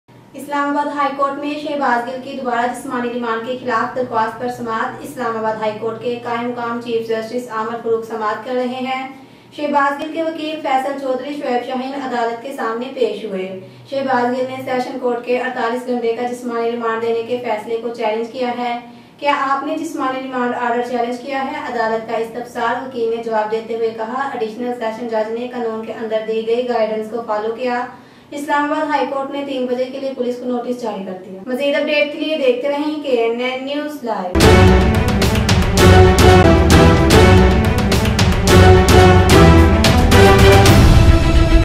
IHC Mai Shahbaz Gill Ki Dobara Jismani Remand K Khilaf Drkhwast Pr Samat#KNN NEWS LIVE 18 Aug 2022
IHC Mai Shahbaz Gill Ki Dobara Jismani Remand K Khilaf Drkhwast Pr Samat